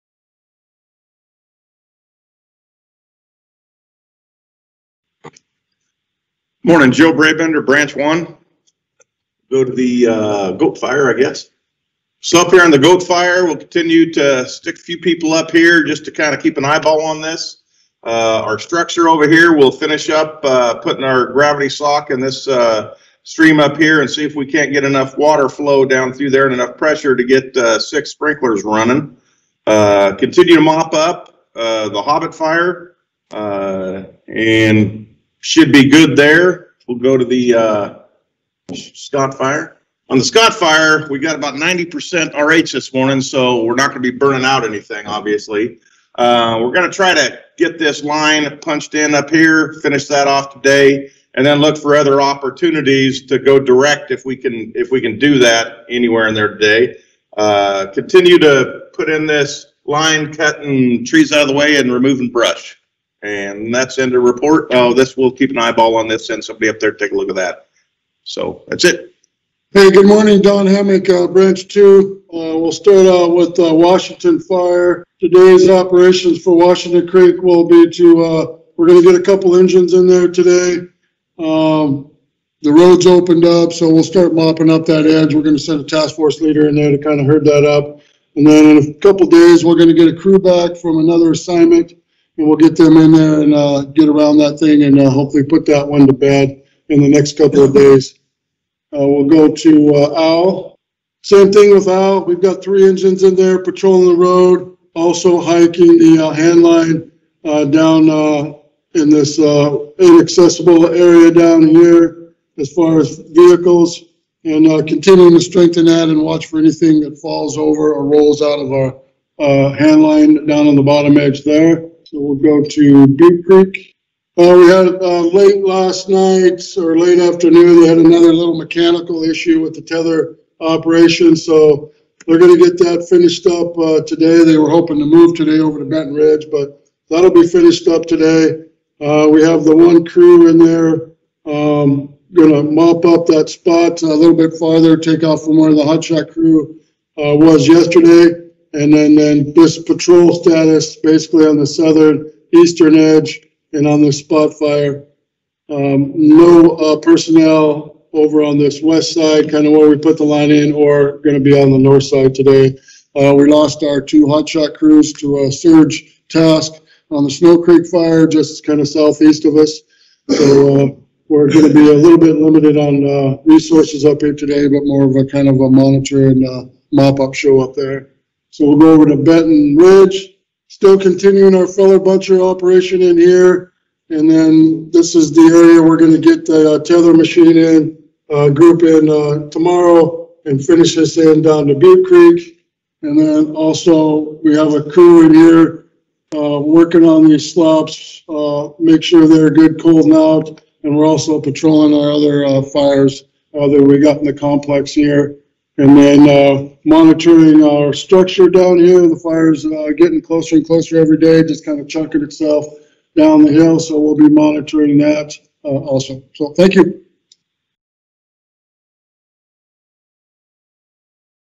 Cougar Rock Complex Morning Briefing for July 21, 2021
Operation Branch Director's give a morning briefing on the status of the Cougar Rock Complex.